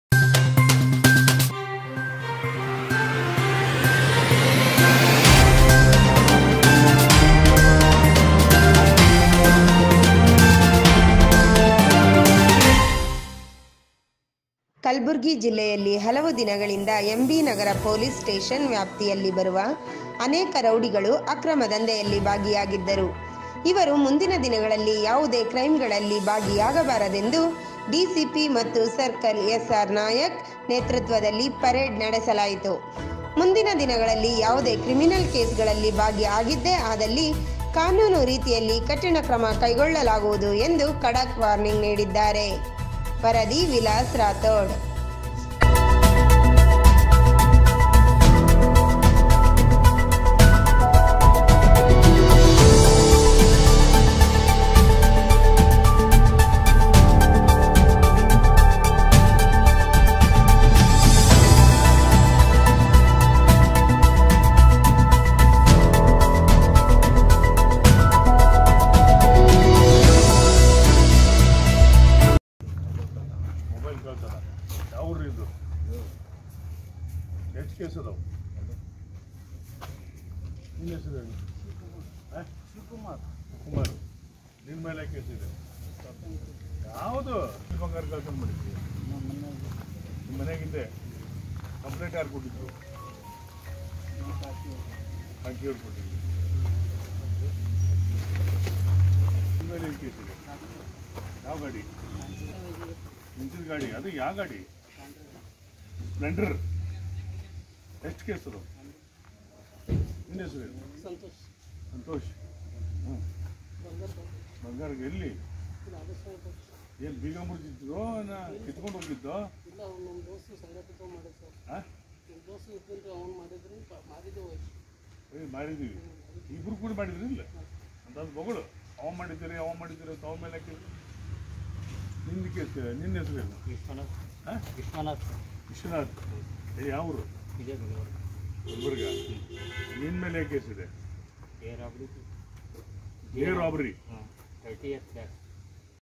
ಎಂ.ಬಿ ನಗರ ಪೋಲಿಸ್ ಸ್ಟೇಷನ್ ನಲ್ಲಿ ರೌಡಿಶಿಟರಗಳ ಪರೇಡ್
ಕಲಬುರ್ಗಿ  ಜಿಲ್ಲೆಯಲ್ಲಿ ಹಲವು ದಿನಗಳಿಂದ. ಎಂ.ಬಿ  ನಗರ ಪೋಲಿಸ್ ಸ್ಟೇಷನ್ ವ್ಯಾಪ್ತಿಯಲ್ಲಿ ಬರುವ ಅನೇಕ ರೌಡಿಗಳು ಅಕ್ರಮ ದಂಧೆಯಲ್ಲಿ ಭಾಗಿಯಾಗಿದ್ದರು , ಇವರು ಮುಂದಿನಗಳಲ್ಲಿ ಯಾವುದೇ ಕ್ರೈಂ ಗಳಲ್ಲಿ, ಭಾಗಿ ಯಾಗಬಾರದೆಂದು ಡಿ.ಸಿ.ಪಿ  ಮತ್ತು ಸರ್ಕಲ್ ಎಸ್ .ಆರ್ ನಾಯಕ ನೇತೃತ್ವದಲ್ಲಿ ಪರೇಡ ನಡೆಸಲಾಯಿತು , ಮುಂದಿನಗಳಲ್ಲಿ ಯಾವುದೇ ಕ್ರಿಮಿನಲ್ ಕೇಸ್ ಗಳಲ್ಲಿ ಭಾಗಿ ಆಗಿದ್ದೆ ಆದಲ್ಲಿ, ಕಾನೂನು ರೀತಿಯಲ್ಲಿ ಕಠಿಣಕ್ರಮ ಕೈಗೊಳ್ಳಲಾಗುವುದು ಎಂದು ಖಡಕ್ ವಾರ್ನಿಗ್ ನೀಡಿದ್ದಾರೆ.